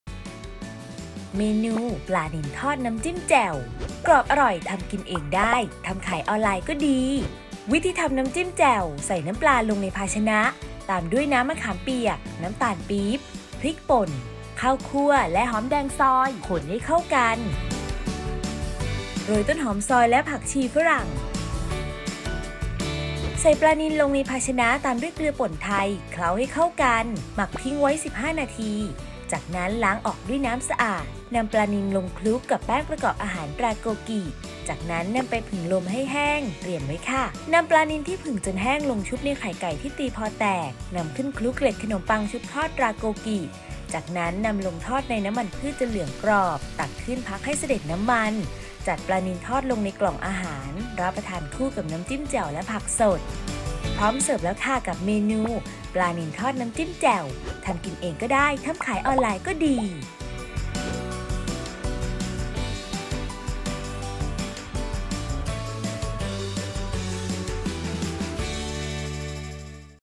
ปลานิลทอดน้ำจิ้มแจ่ว สูตรเด็ด ทำง่าย สไตล์ อ.ยิ่งศักดิ์
ปลานิลทอด น้ำจิ้มแจ่ว (ผลิตภัณฑ์ แป้งประกอบอาหาร โกกิ,เกล็ดขนมปังชุบทอด โกกิ)

สิ่งที่ต้องเตรียม (น้ำจิ้มแจ่ว)
น้ำตาลปี๊บ                                                       2    ช้อนชา
พริกป่น                                                           2    ช้อนชา
ข้าวคั่ว                                                            2    ช้อนชา
ต้นหอมซอยและผักชีฝรั่งซอย สำหรับโรยหน้า
สิ่งที่ต้องเตรียม (ปลานิลทอด)
ปลานิล (ขอดเกล็ดควักไส้ออก) บั้ง หนัก 300 กรัม    1   ตัว
เกลือป่นไทย                                                       20   กรัม
แป้งประกอบอาหาร ตราโกกิ                              1   ซอง
เกล็ดขนมปังชุบทอด ตราโกกิ                            100   กรัม
ผักสด (ผักชีฝรั่ง ,ผักกาดหอม ,ผักชี,ผักกาดขาว,กรีนโอ๊ค,เร้ดโอ๊ค) สำหรับรับประทานคู่

วิธีทำ (น้ำจิ้มแจ่ว)
1. ใส่น้ำปลาลงในภาชนะ ตามด้วยน้ำมะขามเปียก น้ำตาลปี๊บ พริกป่น ข้าวคั่วและหอมแดง ให้เข้ากัน
2. โรยต้นหอมซอยและผักชีฝรั่งซอย ตักใส่ถ้วยน้ำจิ้มที่มีฝาปิด เตรียมไว้
วิธีทำ (ปลานิลทอด)
1. ใส่ปลานิลลงในภาชนะ ตามด้วยเกลือป่นไทย เคล้าให้เข้ากัน หมักทิ้งไว้ 15 นาที จากนั้นล้างออกด้วยน้ำสะอาด
2. นำปลานิลคลุกลงกับแป้งประกอบอาหาร ตราโกกิ จากนั้นนำไปผึ่งลมให้แห้ง เตรียมไว้
3. นำปลานิลที่ผึ่งจนแห้งลงชุบในไข่ไก่ตีพอแตก นำขึ้นคลุกเกล็ดขนมปังชุบทอด ตราโกกิ จากนั้นนำลงทอดในน้ำมันพืชจนเหลืองกรอบ ตักขึ้น พักให้สะเด็ดน้ำมัน
4. จัดปลานิลทอดลงในกล่องอาหาร รับประทานคู่กับน้ำจิ้มแจ่วและผักสด

▲Line @yingsakfood